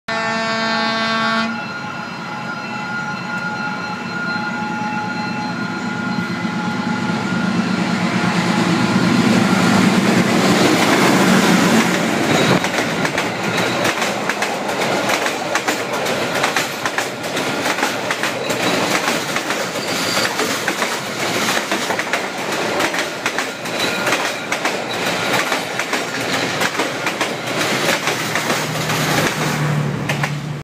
[26/01/2019] D13E-710 kéo tàu SE26 qua chắn Tô Ngọc Vân
Chiến dịch tàu tết Xuân Kỷ Hợi 2019
Tàu SE26 : Sài Gòn đi Quãng Ngãi